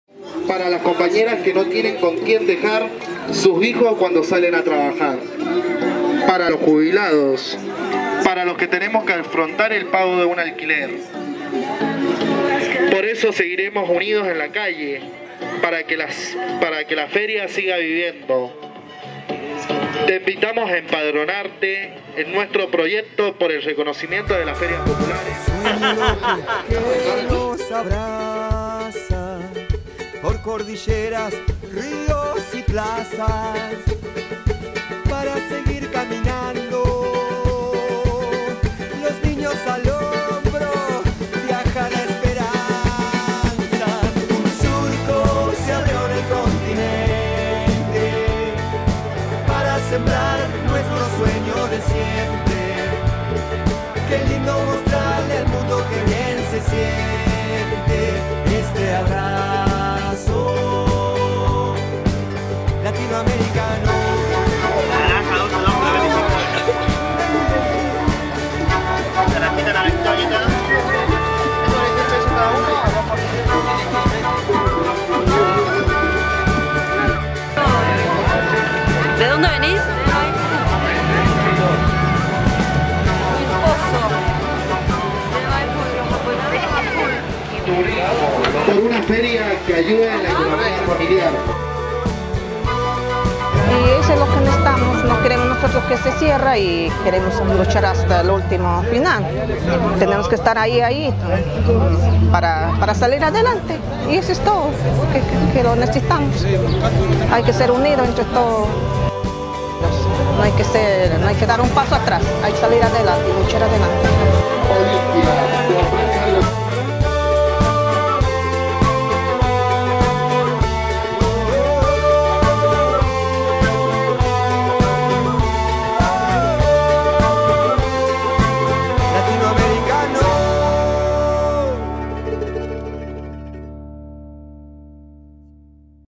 No al desalojo de la feria popular de Guaymallén - Noviembre 2016
Ante las amenazas de desalojo de la feria popular que se realiza sábados y domingo en el predio del Ferrocarril Belgrano en San José, Guaymallén, vecinos y feriantes defienden su mercado y fuentes de trabajo. 6 de Noviembre de 2016.